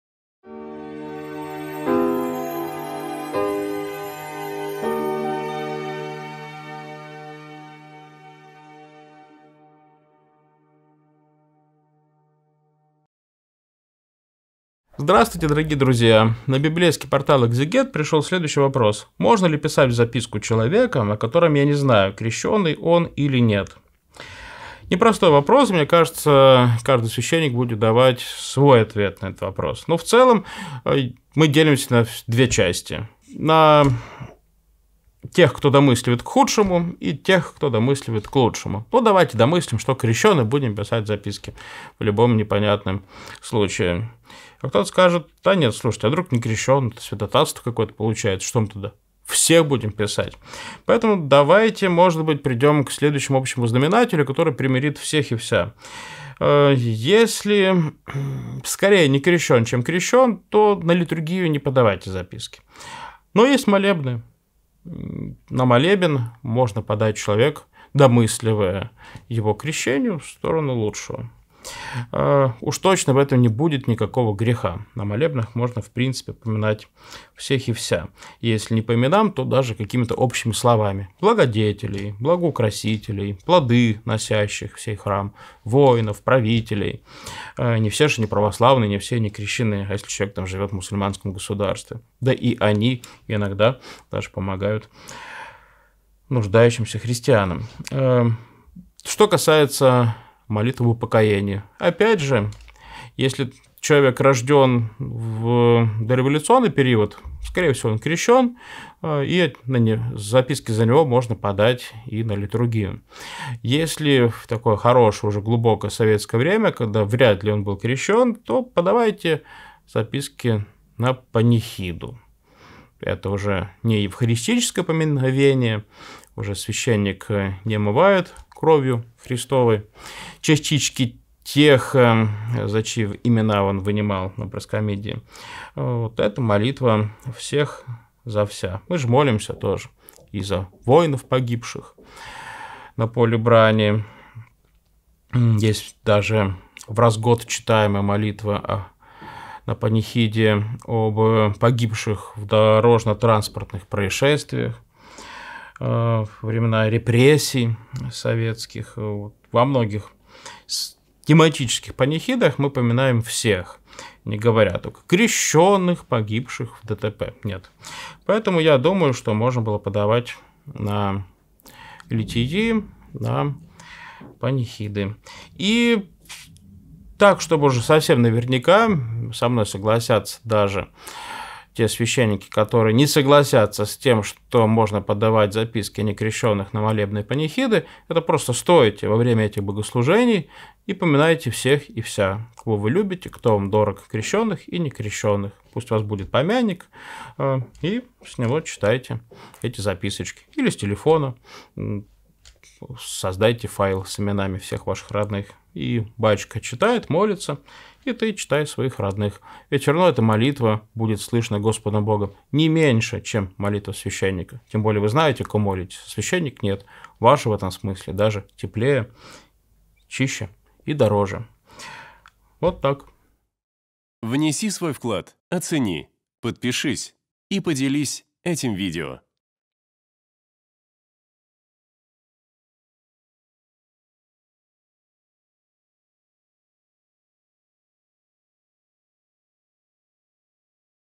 МОЖНО ЛИ ПОДАВАТЬ ЗАПИСКУ, ЕСЛИ НЕ УВЕРЕН, ЧТО ЧЕЛОВЕК КРЕЩЕН ? Священник Владислав Береговой
Команда Экзегет.

На библейском портале Экзегет вместе со священником Владиславом Береговым отвечаем на вопрос: "Можно ли подавать записку, если не уверен, что человек крещен?"

🔸  Молитва на молебне
🔸  Поминовение о упокоении на панихиде
🔸  Личная молитва